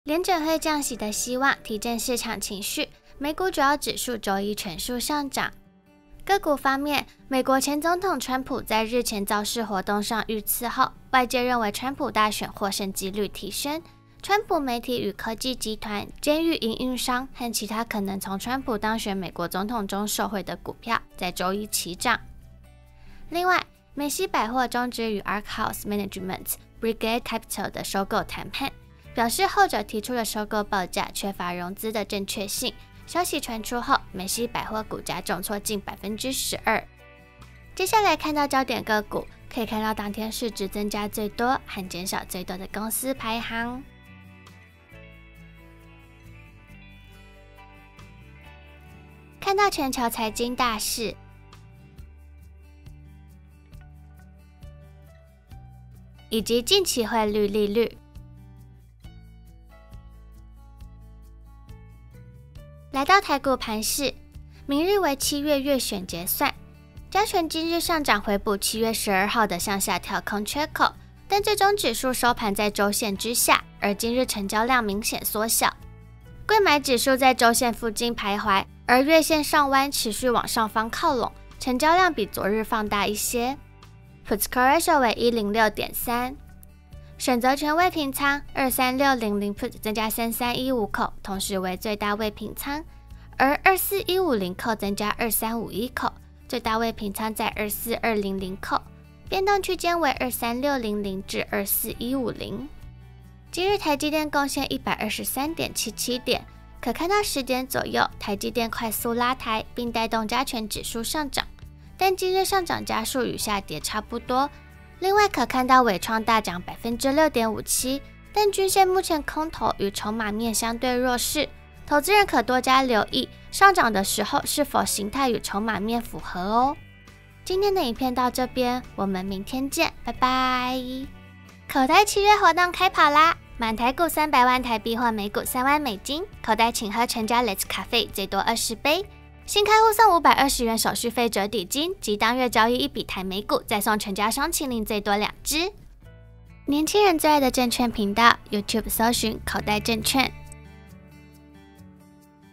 緯創(3231)大漲逾6%，受惠AI動能加持！川普造勢遇刺，相關概念股週一上漲！| 口袋日報 | 2024.07.16
0:07 與川普有關類股週一上漲
1:07 加權07/16盤勢變化
1:16 櫃買07/16盤勢變化
1:43 台積電(2330)盤勢變化
1:55 緯創(3231)盤勢變化
2:11 新開戶就送520手續費折抵金❤️

🔥7月新戶好禮，新開戶送【520】手續費折抵金！

⚠️節目內容僅供參考，投資人應獨立判斷
本節目內提到的個股標的僅供參考，投資人應獨立判斷，審慎投資自負風險